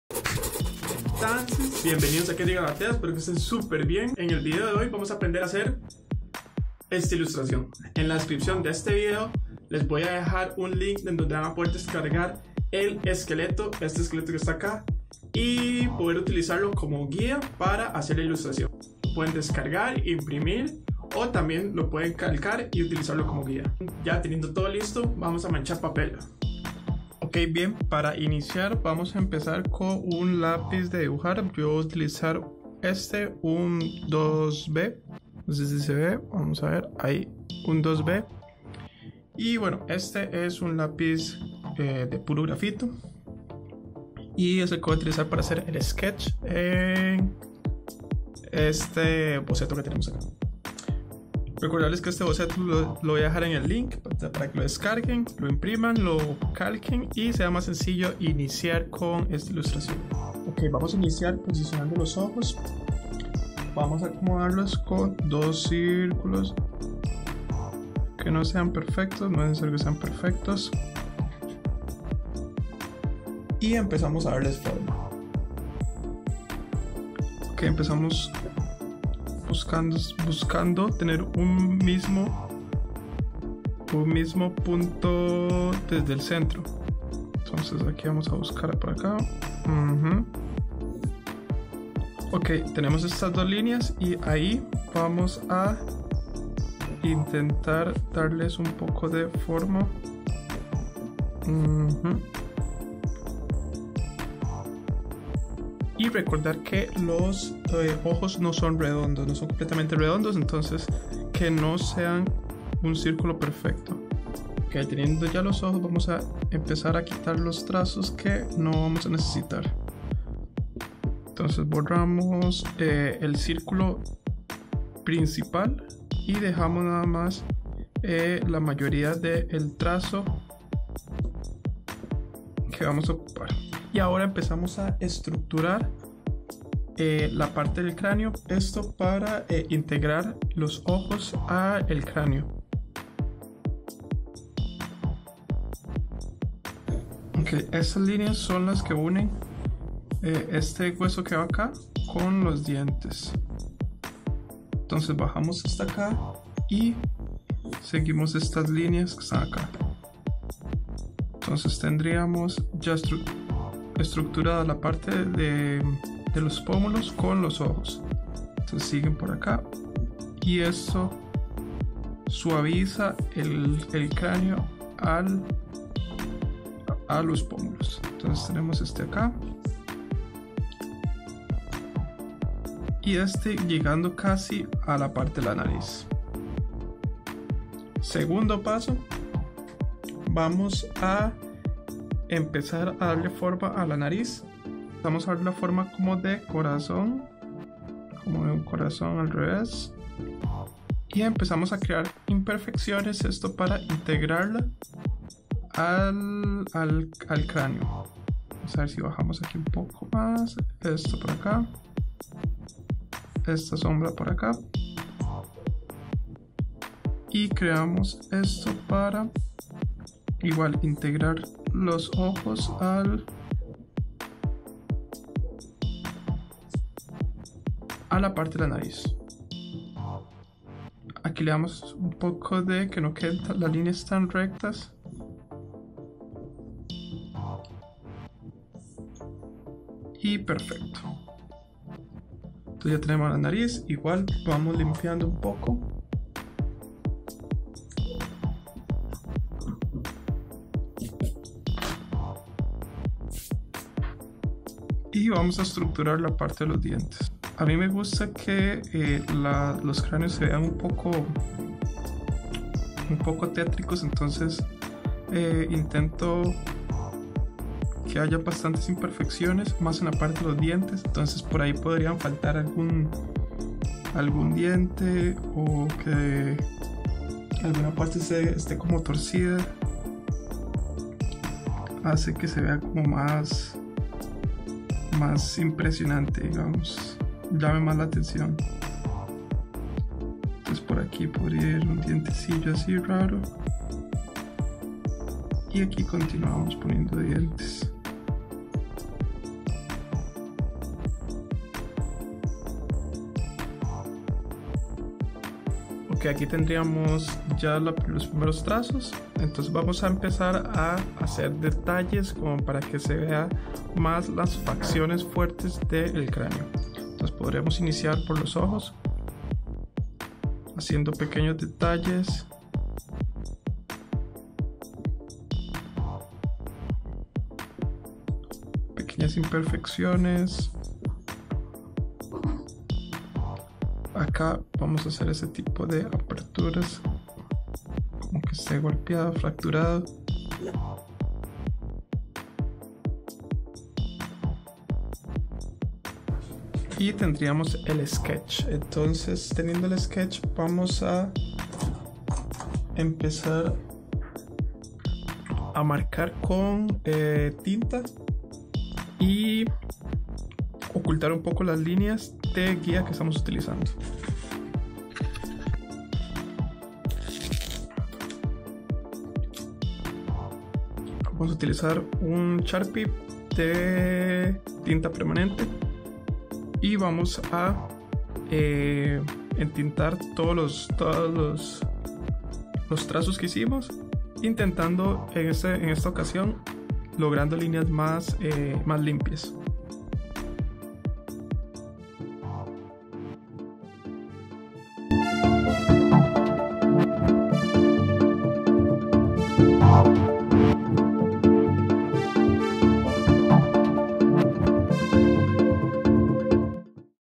Pintemos una Catrina botánica - Parte .1
Hagamos una ilustración botánica, agregando una catrina color naranja súper llamativa, perfecta para aprender en casa
Parte 1 - aprenderemos técnicas para hacer el boceto antes de pintar
Parte 2 - Aplicaremos la técnica húmedo sobre seco y daremos color a la ilustración

Les comparto las líneas guías perfecto para iniciar a crear